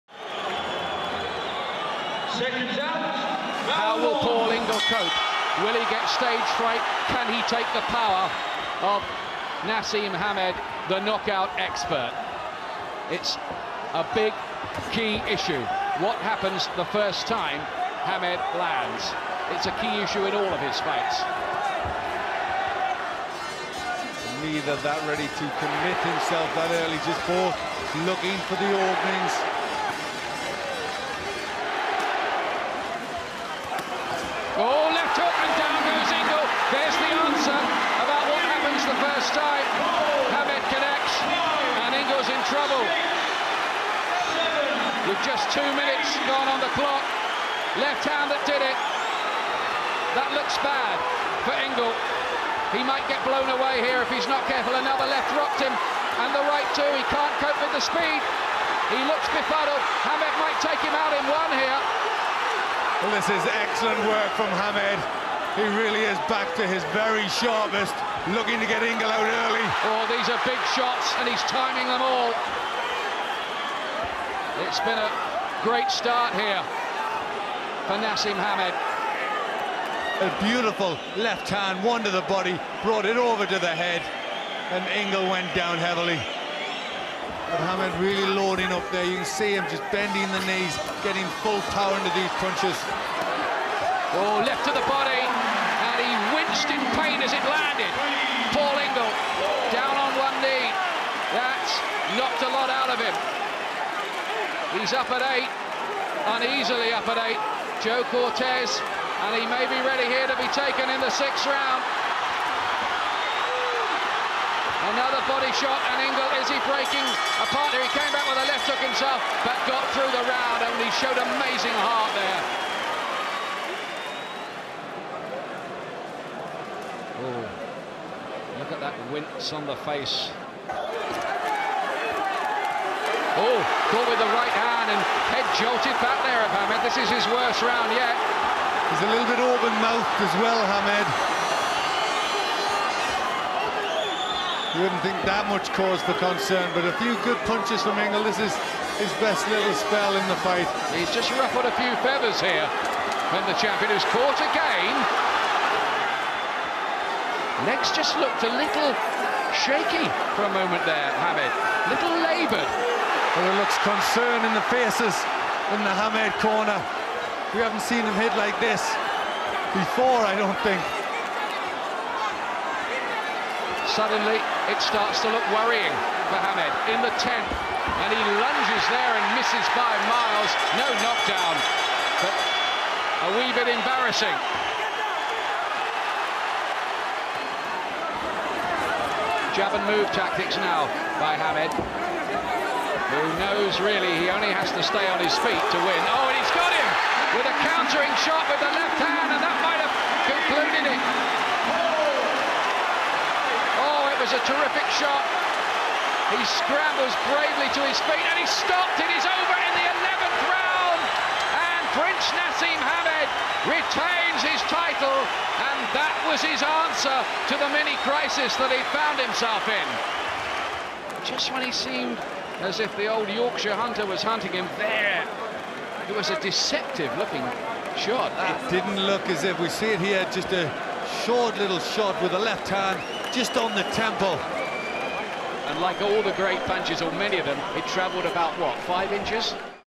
'Prince' Naseem Hamed's EPIC ring walk and knockout against Paul Ingle! 💥
In 1999, Naseem Hamed arrived in style and then beat British rival Paul Ingle in even more style...